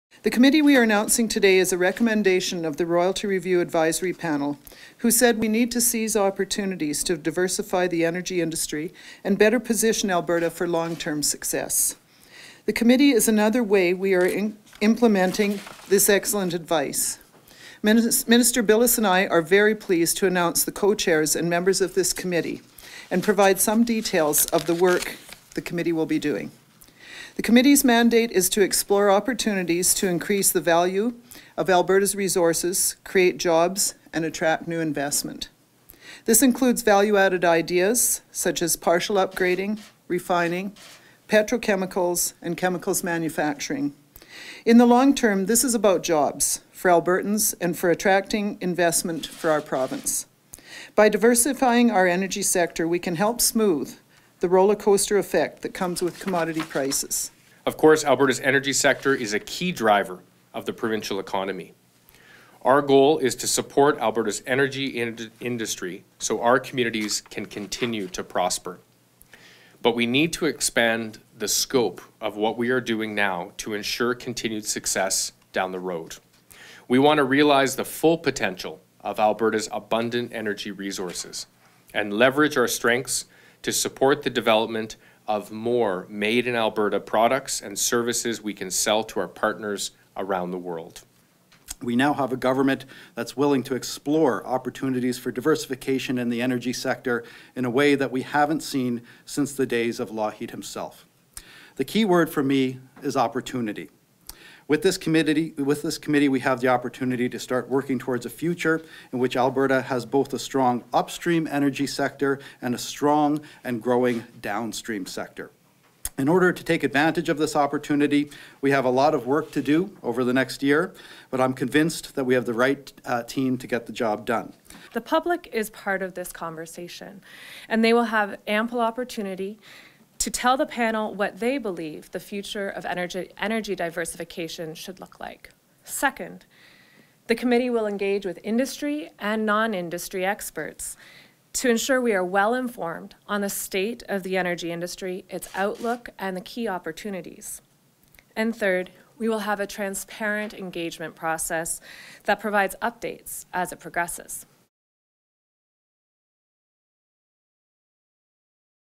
Energy diversification advisory committee named
Energy Minister Margaret McCuaig-Boyd and Economic Development and Trade Minister Deron Bilous announce the members of the newly appointed Energy Diversification Advisory Committee, whose role is to explore opportunities for increasing value-added development in Alberta. The creation of this committee was a recommendation made by Alberta’s Royalty Review Advisory Panel.